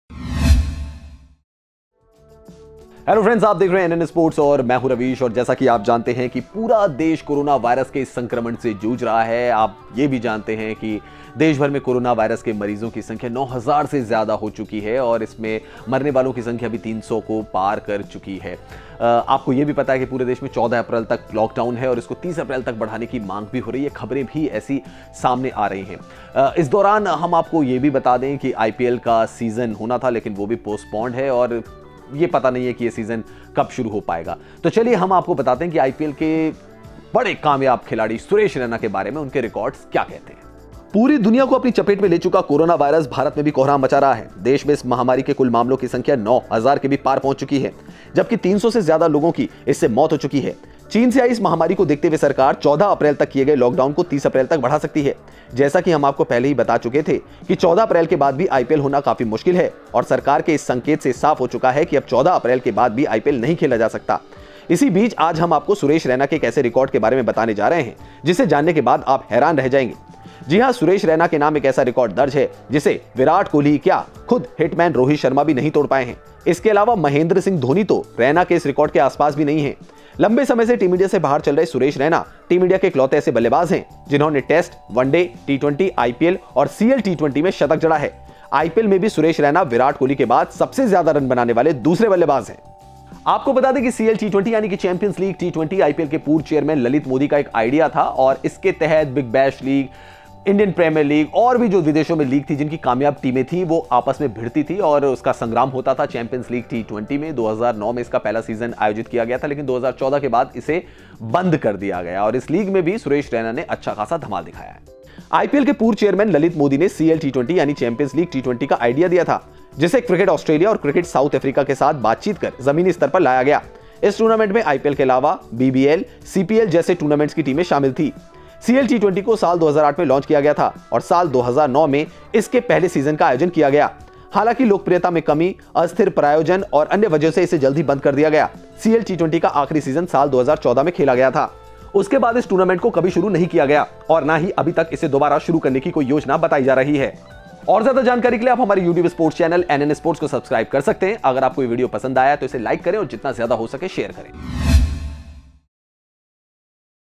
जो काम विराट और धोनी नहीं कर पाए, उसे रैना ने कर दिखाया
|India|Lockdown|IPL|Suresh Raina|Cricket|CLT20|

India में महामारी का कहर दिन-प्रतिदिन बढ़ता जा रहा है. देश में अब महामारी के कुल मामलों की संख्या 9 हजार के भी पार पहुंच चुकी है और इस महामारी से मरने वालों का आंकड़ा भी 300 से ज्यादा हो गया है. चीन से आई इस महामारी को देखते हुए सरकार 14 अप्रैल तक किए गए Lockdown को 30 अप्रैल तक बढ़ा सकती है. जैसा कि हम आपको पहले ही बता चुके थे कि 14 अप्रैल के बाद भी IPL होना काफी मुश्किल है. इसी बीच आज हम आपको Suresh Raina के एक ऐसे रिकॉर्ड के बारे में बताने जा रहे हैं, जिसे जानने के बाद आप हैरान रह जाएंगे.